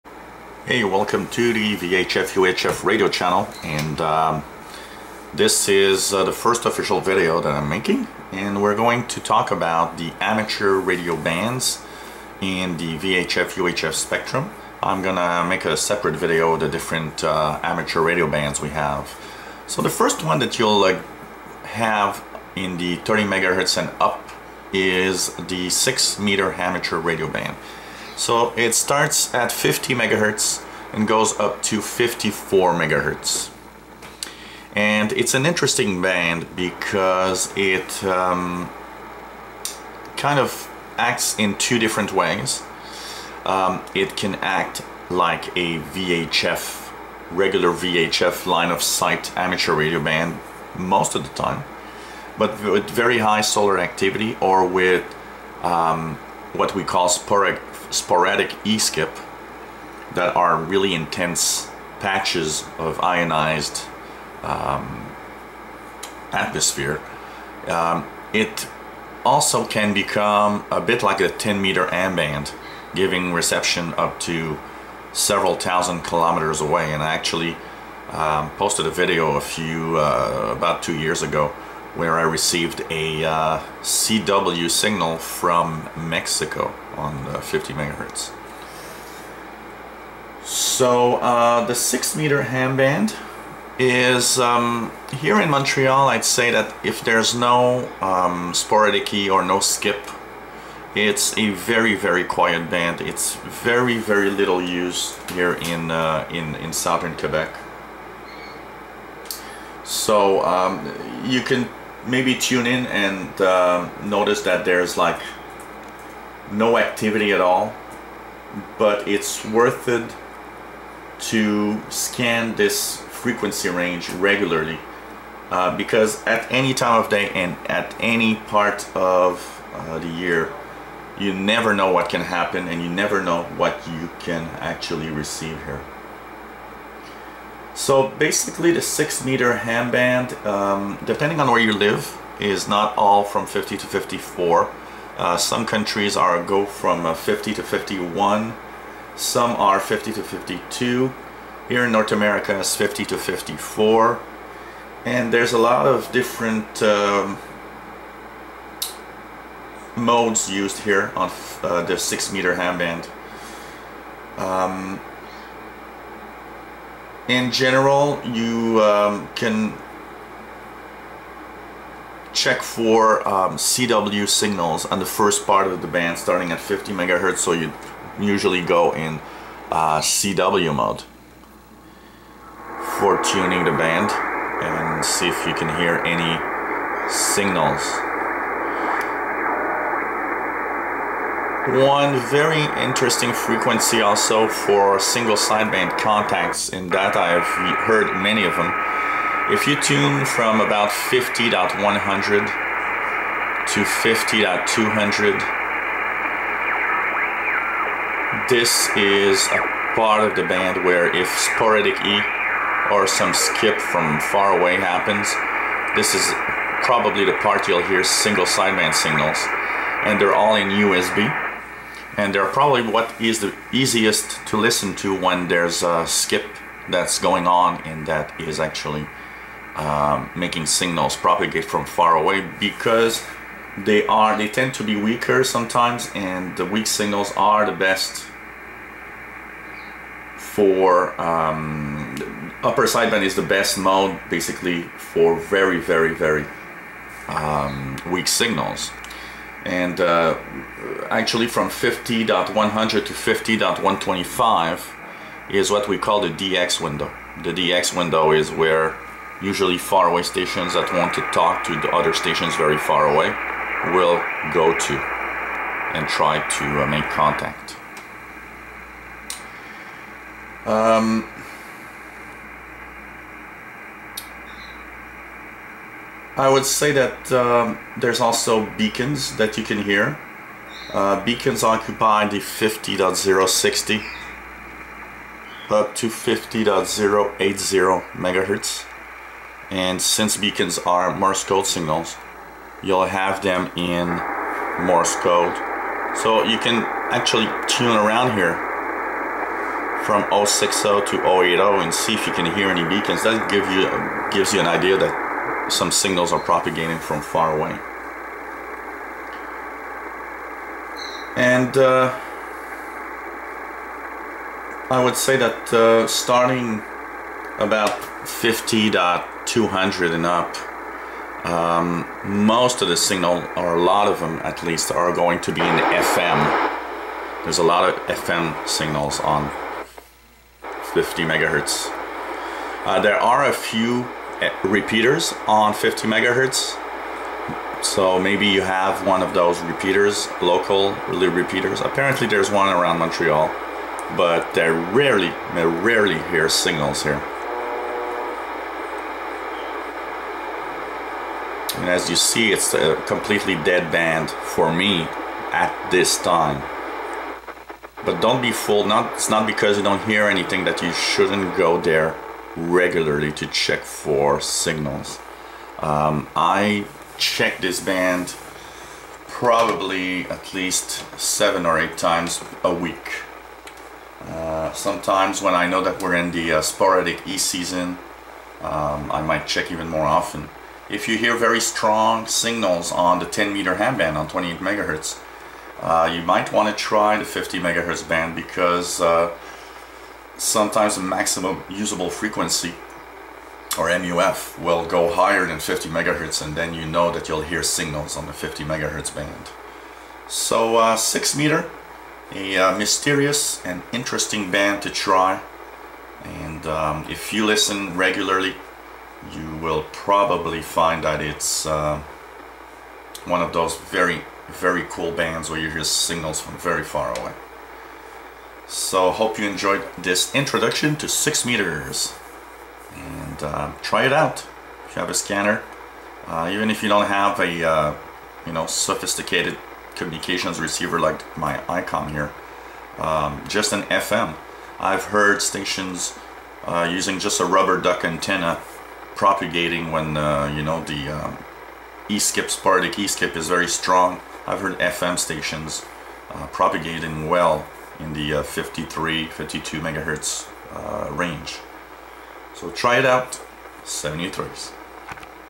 Introduction to the 6 meter amateur radio band 50 to 54 Mhz
6 meter ham band explanation